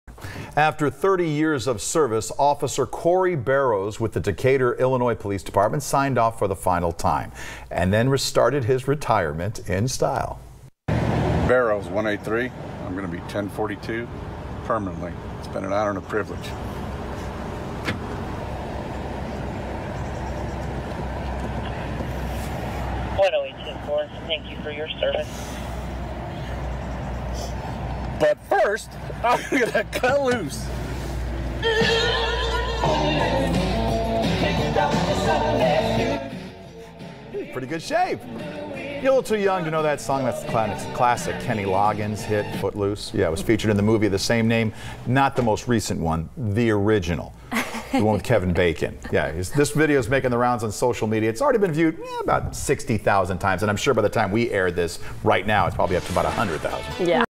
Illinois officer retires in style
After 30 years of service Officer Cory Barrows with the Decatur, Illinois Police Department signed off for the final time. And he started his retirement in style.